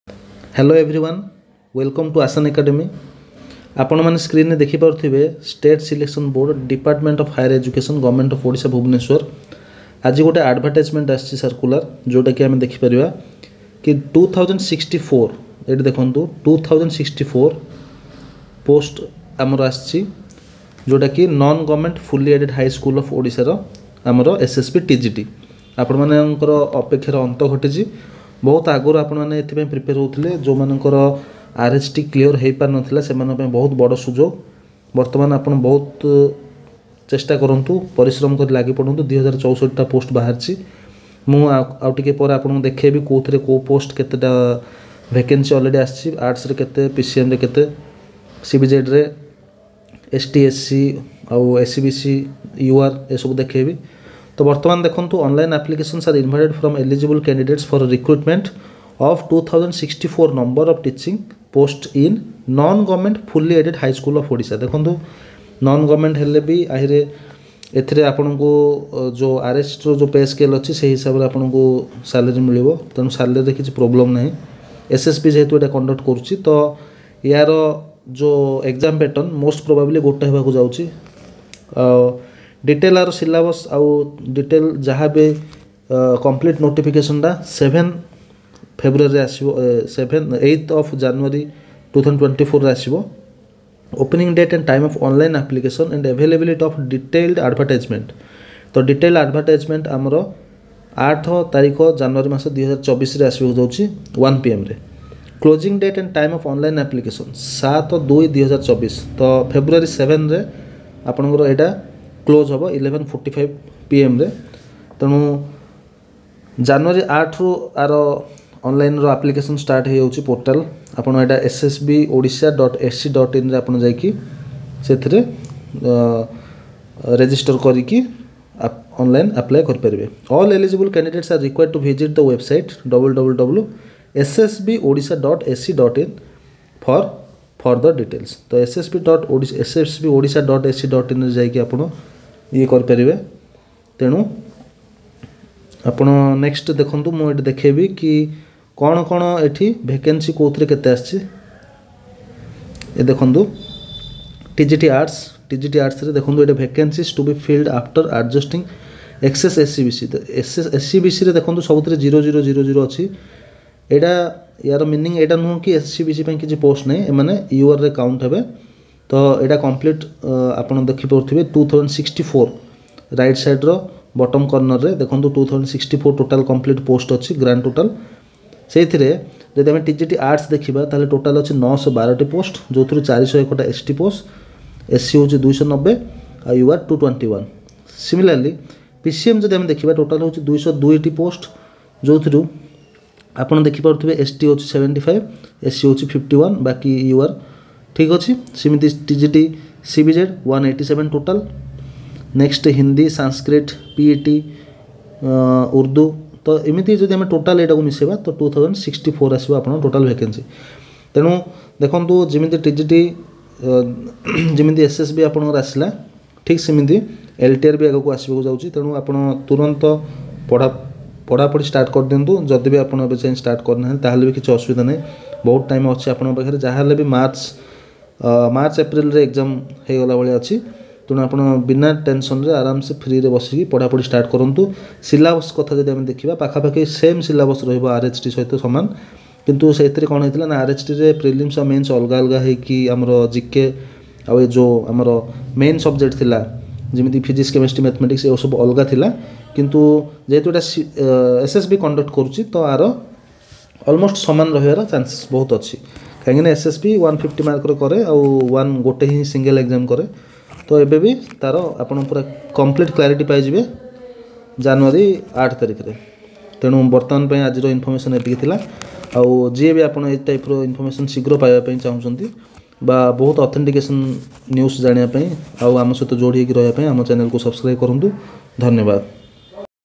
SSB TGT NOTIFICATION ADVERTISEMENT ଆସିଗଲା | ଶୀଘ୍ର ଦେଖନ୍ତୁ | RHT | LTR 2024
This video contains information and advertisement about SSB TGT 2024 notification.